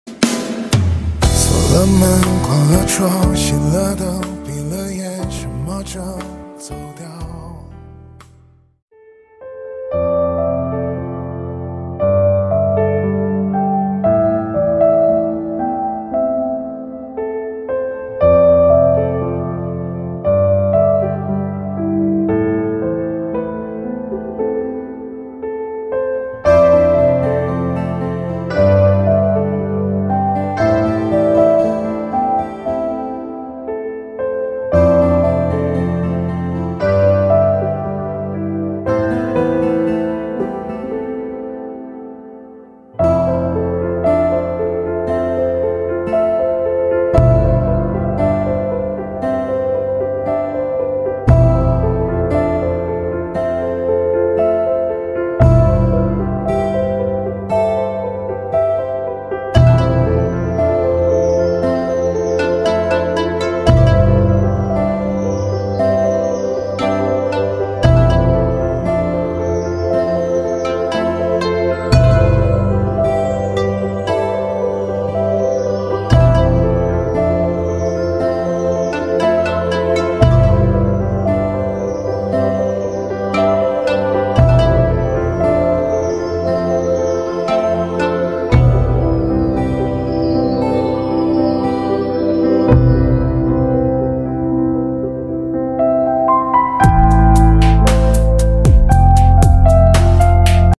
林妹一个人种土豆！希望秋天能有一个好收成！【寻山记】
大凉山的彝族风情！大凉山
TIKTOK Chinese Food#Chinese costume#minority#eleven#belle
往期精彩视频：